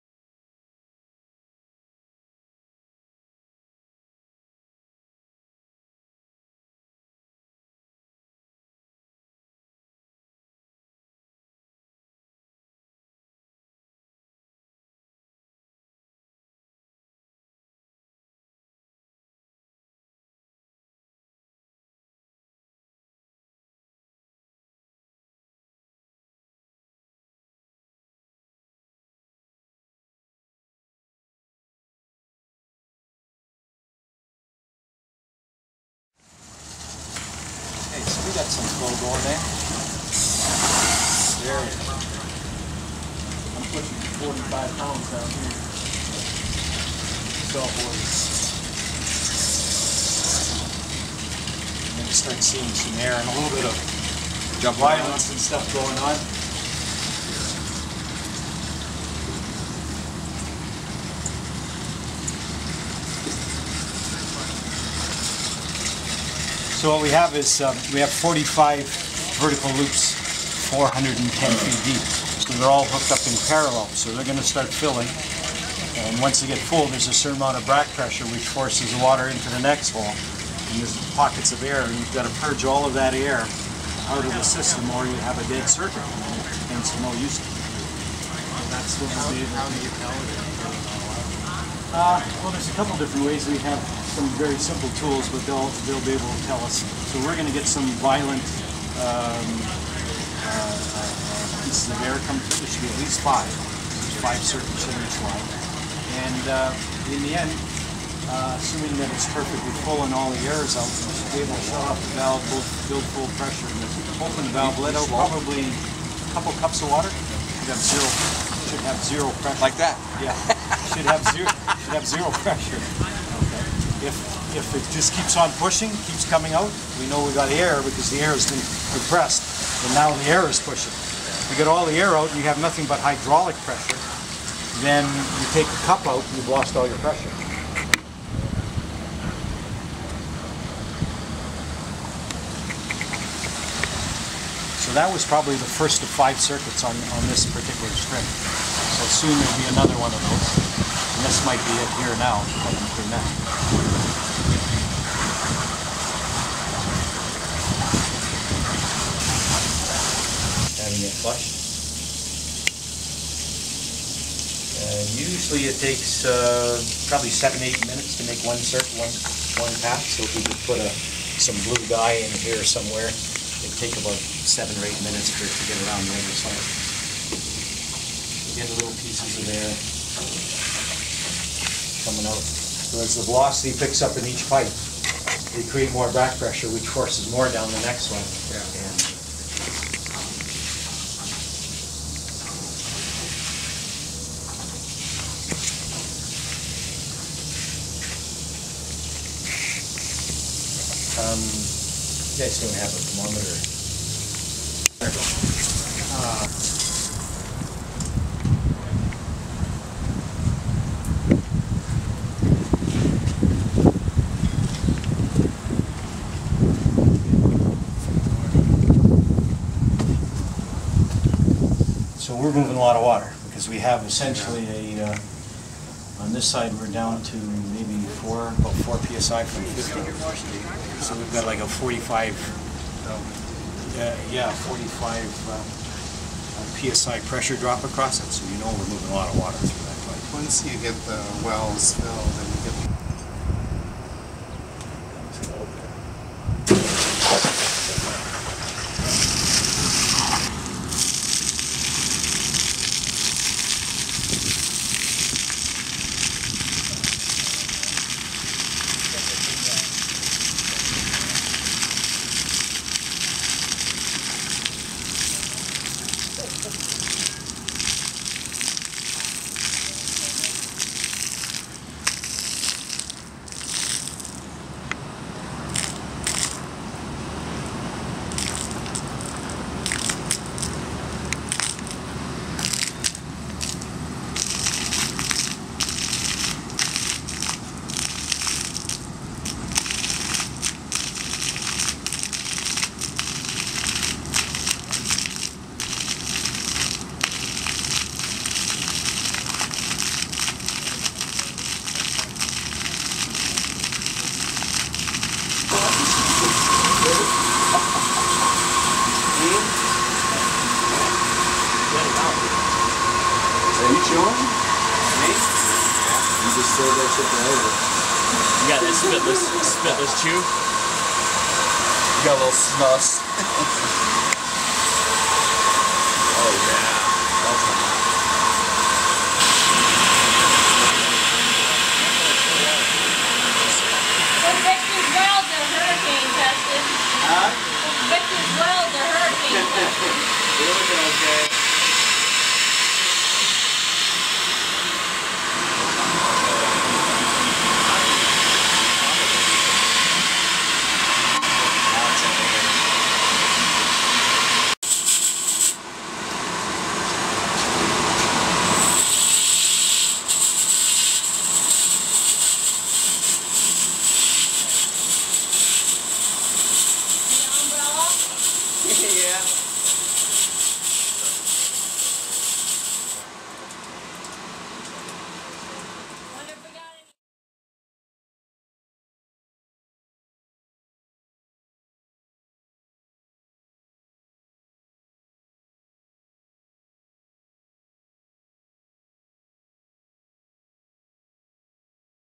Plymouth Ice Rink Geothermal Loop Flush
GeoSmart Energy CEO, Stan Marco, flushes the geothermal loop that will provide the heating and cooling for the Plymouth Cultural Centre in Plymouth, Michigan. Even the ice rink ice will be made with the geothermal system. GeoSmart Energy's Geothermal Specialist, Thermo Source, was the geothermal contractor for the project which was completed in 2010.

For more information on how geothermal can heat and cool your home, business, farm or even ice rink, visit: